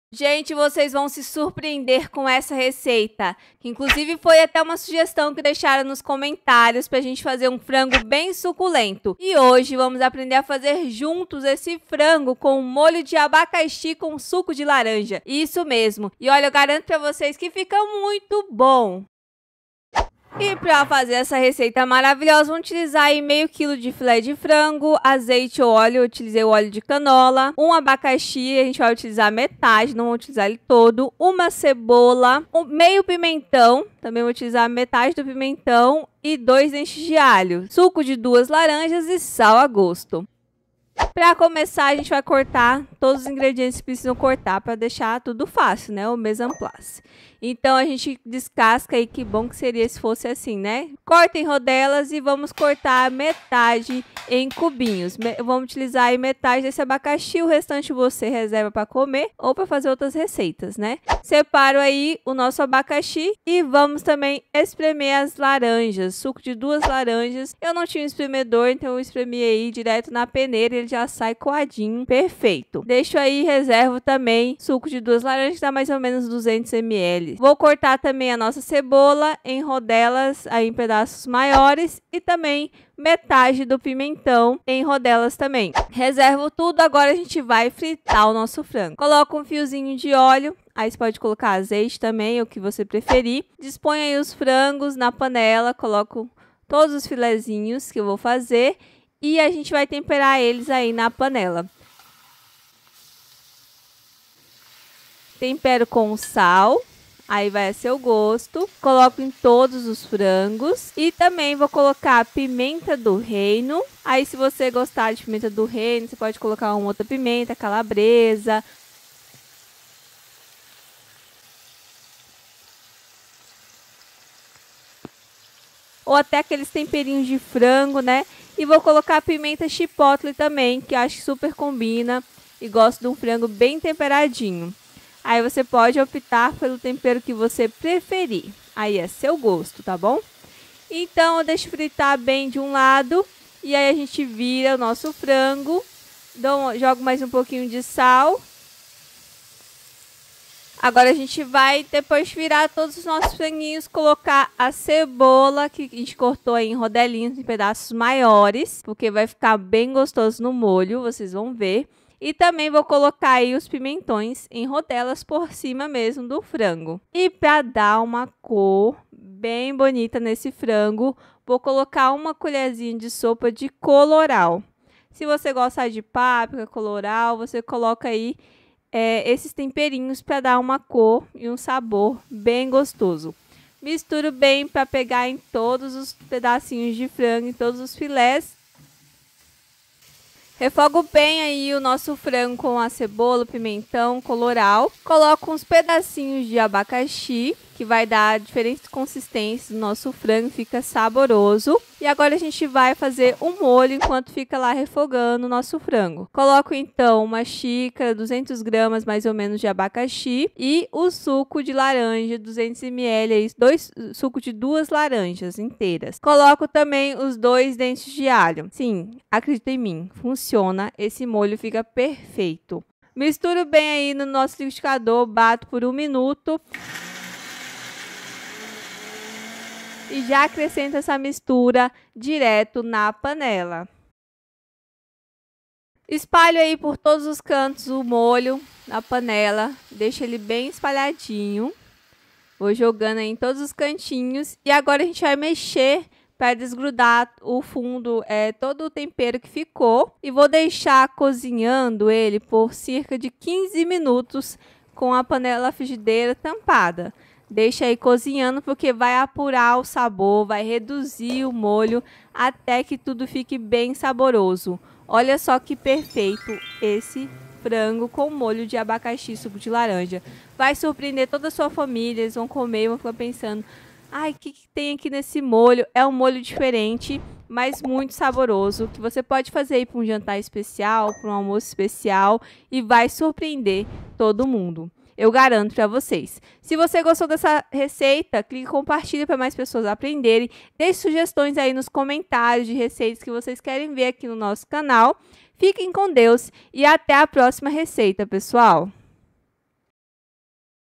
FILÉ DE FRANGO AO MOLHO DE ABACAXI
Oi gente no vídeo de hoje vamos fazer juntos um filé de frango ao molho de abacaxi com suco de laranja super suculento, diferente e saboroso bem fácil de fazer!
Esse molho é surpreendente e dá todo um toque especial ao frango, transformando sua refeição em algo muito especial!

Link da Frigideira

Ingredientes:

500g de filé de frango
200g de abacaxi
200ml de suco de laranja
2 dentes de alho
1 cebola
1/2 pimentão
1 colher de colorau
pimenta chipotle à gosto

Tempo de Preparo: 30min
Tempo de Cozimento do molho: 15min

"Porque quem cozinha com amor nunca perde o tempero e nem esquece o sabor"
Paulo E. Soares